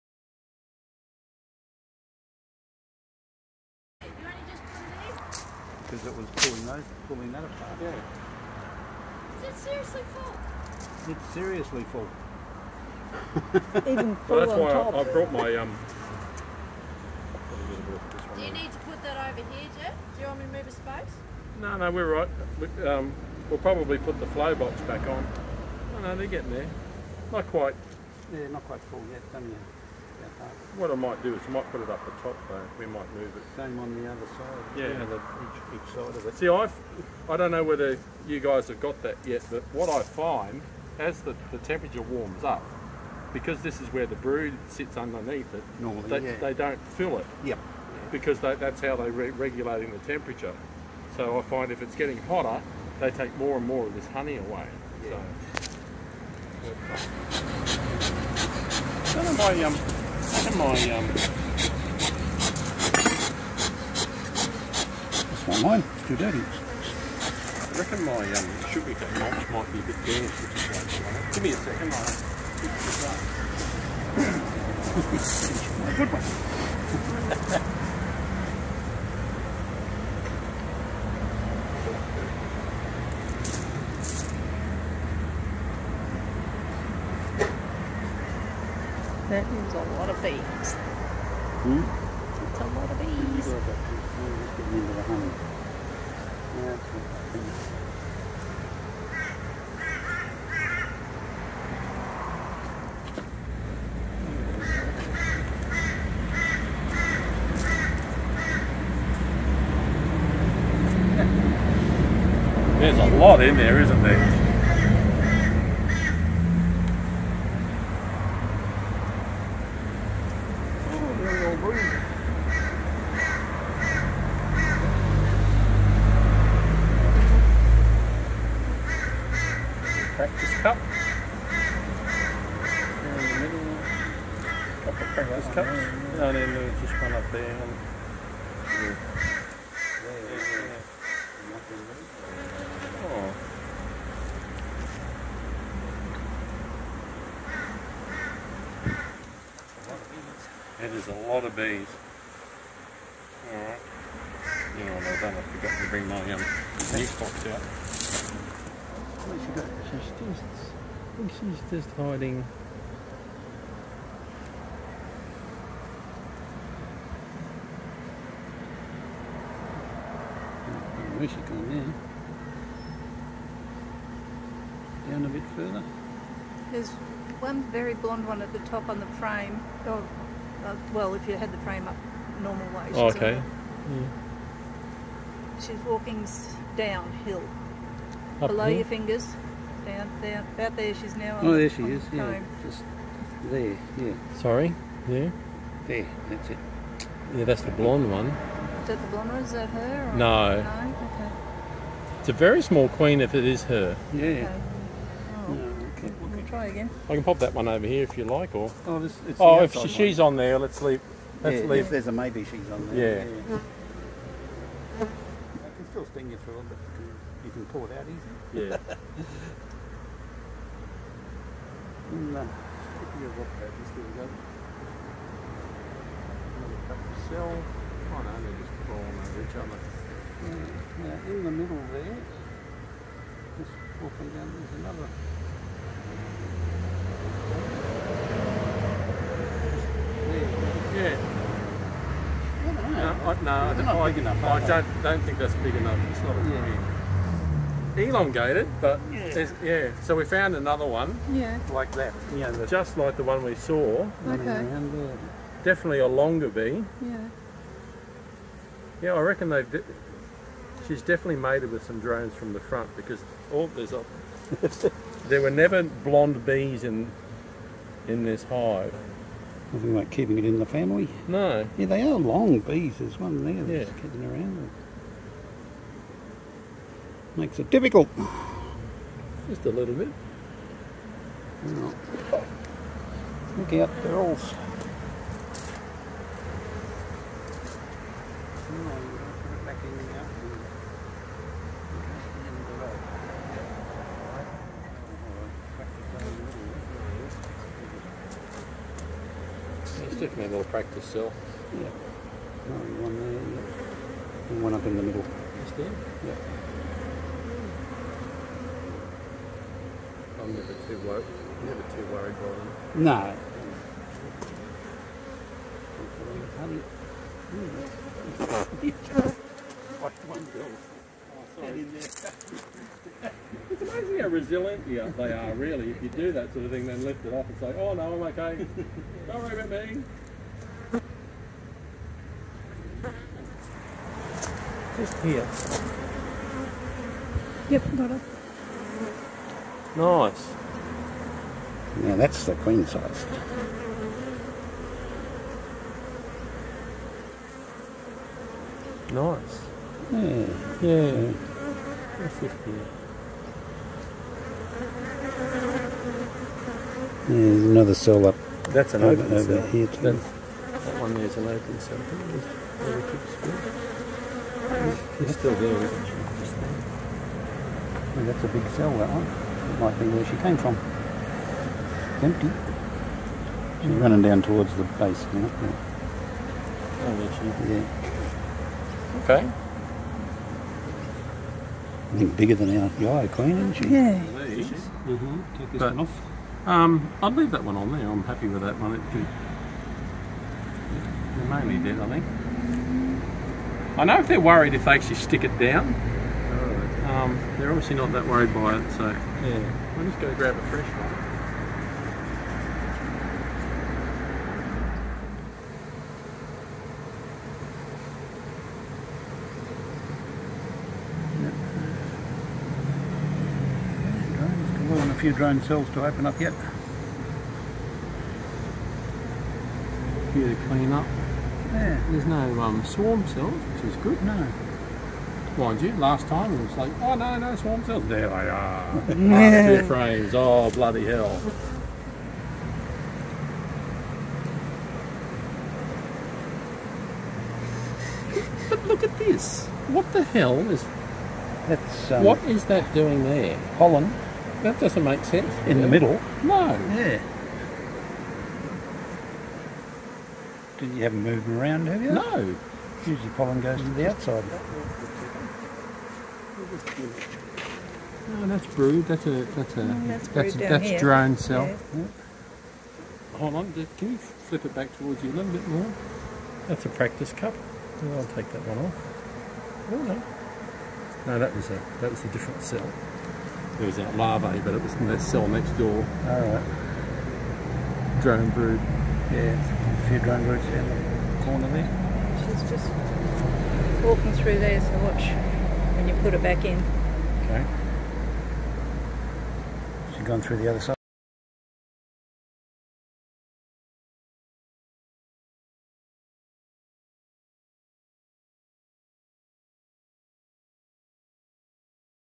The BEE Team.
Today we helped out Jeff from 4152 Honey (on Facebook) with his inspections.  Jeff had a recent injury and cannot lift heavy boxes for a while.
Carina Honey to the rescue!
This is a few clips from the day.